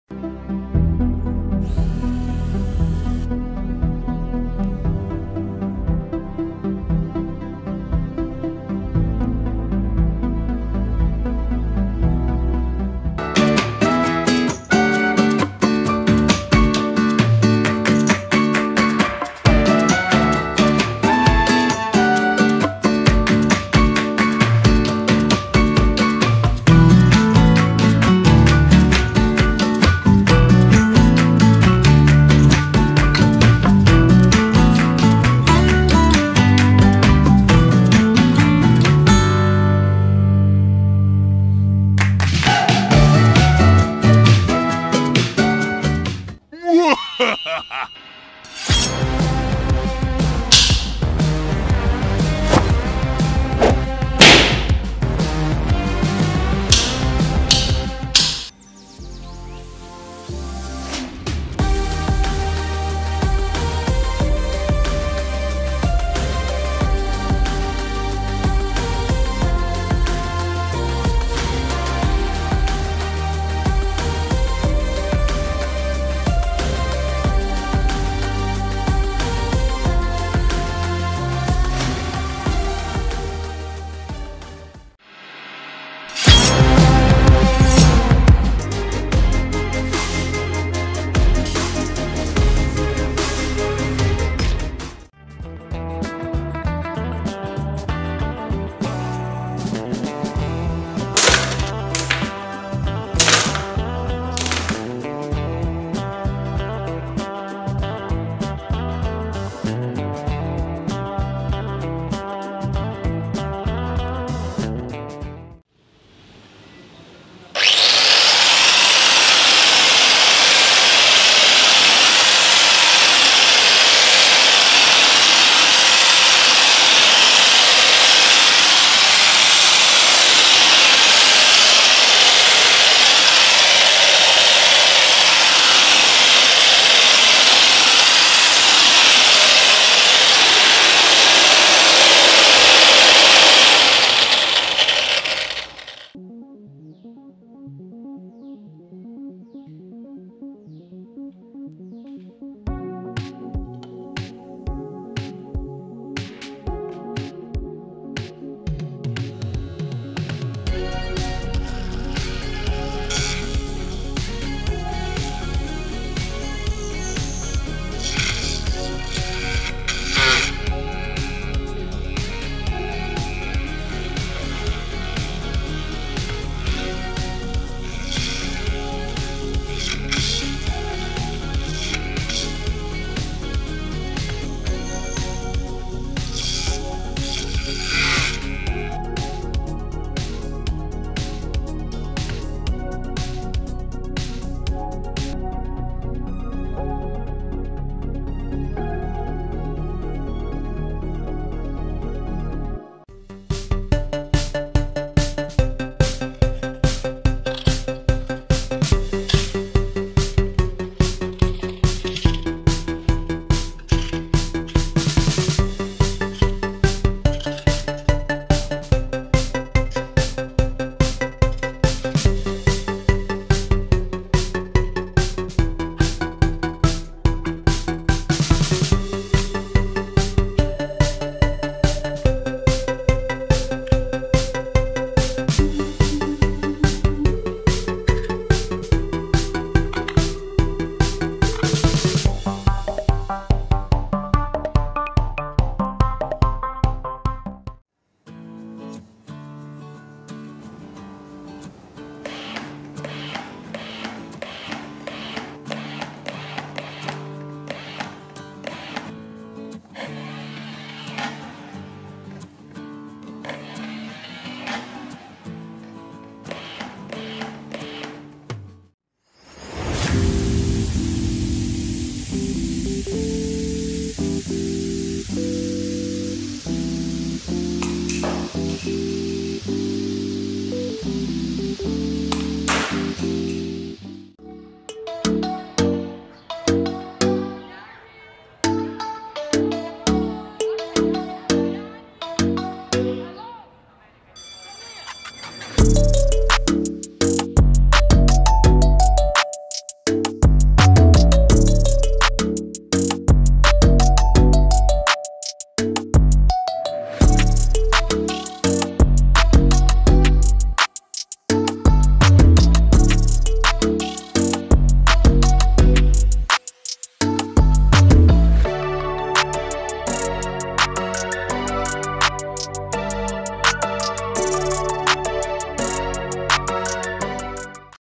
Top 10 Genuis ideas of 2022
In this video I show you Genuis ideas Of 202 , it's quite fun to play with. all Projects Build So Simple easy to make at Home..

I hope you liked this video - Share this with your friends.